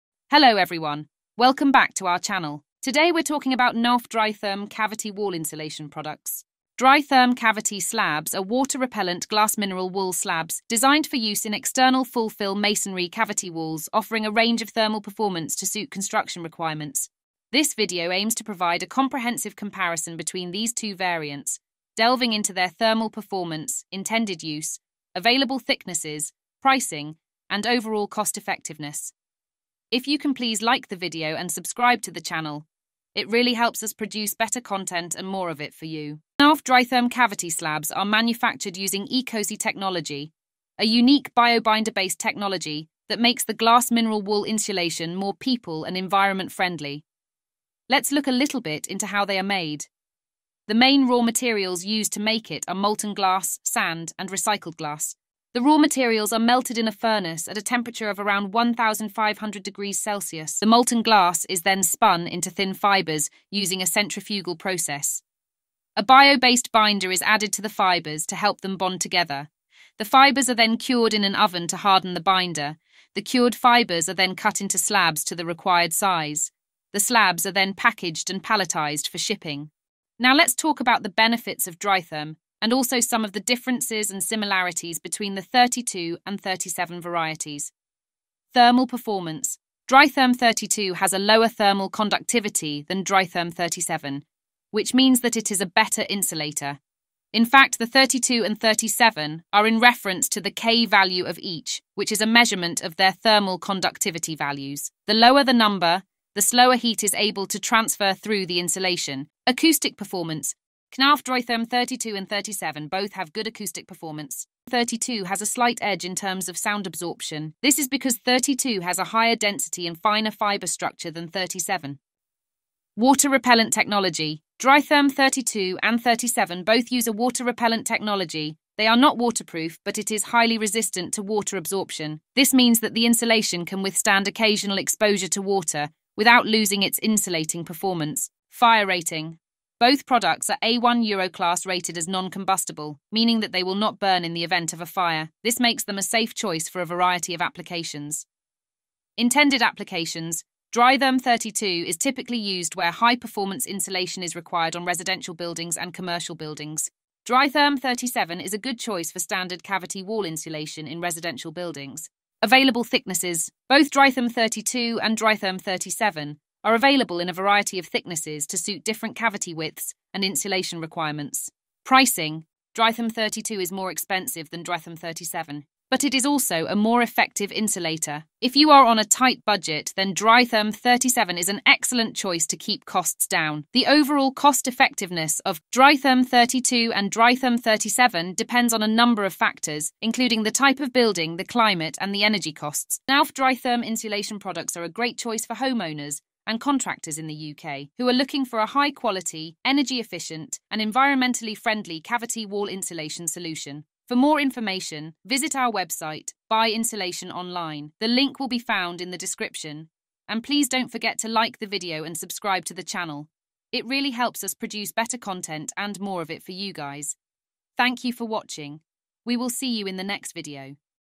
Knauf Dritherm Cavity Wall Insulation: A Comprehensive Comparison of 32 and 37
Knauf Dritherm Cavity Wall Insulation is a water-repellent glass mineral wool insulation that is designed for use in external full-fill masonry cavity walls. It is available in two variants: Knauf Dritherm 32 and Knauf Dritherm 37.

In this video, we provide a comprehensive comparison of these two variants, delving into their thermal performance, intended use, available thicknesses, pricing, and overall cost-effectiveness.

Key takeaways:

1. Knauf Dritherm 32 has a lower thermal conductivity than Knauf Dritherm 37, which means that it is a better insulator.
2. Knauf Dritherm 32 has a slight edge over Knauf Dritherm 37 in terms of sound absorption.
3. Both Knauf Dritherm 32 and Knauf Dritherm 37 use a water-repellent technology, but they are not waterproof.
4. Both Knauf Dritherm 32 and Knauf Dritherm 37 are A1 Euroclass rated as non-combustible.
5. Knauf Dritherm 32 is typically used where high-performance insulation is required, while Knauf Dritherm 37 is a good choice for standard cavity wall insulation.
6. Knauf Dritherm 32 is more expensive than Knauf Dritherm 37, but it is also a more effective insulator.
7. The overall cost-effectiveness of Knauf Dritherm 32 and Knauf Dritherm 37 depends on a number of factors, but Knauf Dritherm 32 can often be a more cost-effective choice in the long run due to its superior insulation capabilities and energy savings.